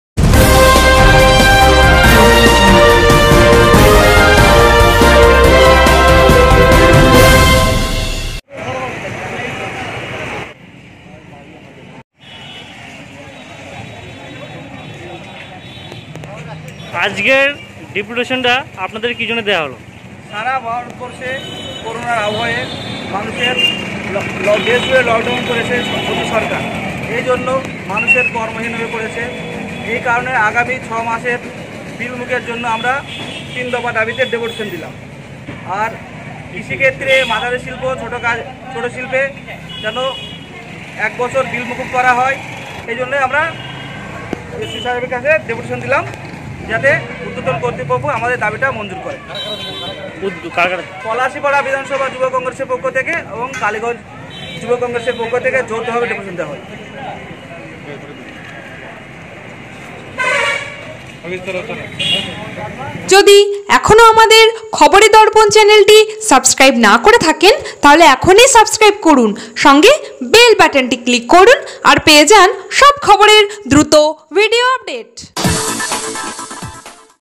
কালিগঞ্জ ও পলাশীপাড়া কংগ্রেসের পক্ষ থেকে তিন দফা দাবিতে কালিগঞ্জ বিদ্যুৎ দপ্তরে ডেপুটেশন
পলাশীপাড়া বিধানসভা ও কালিগঞ্জ বিধানসভা ছাত্র পরিষদের যৌথ উদ্যোগে ও
পলাশীপাড়া ও কালিগঞ্জ বিধানসভা যুব কংগ্রেসের পক্ষ থেকে কালিগঞ্জ বিদ্যুত আধিকারিকে বিদ্যুতের মাসুল কমাতে ও আগামী 6মাস বিল মুকুবের দাবিতে ডেপুটেশন দেওয়া হল।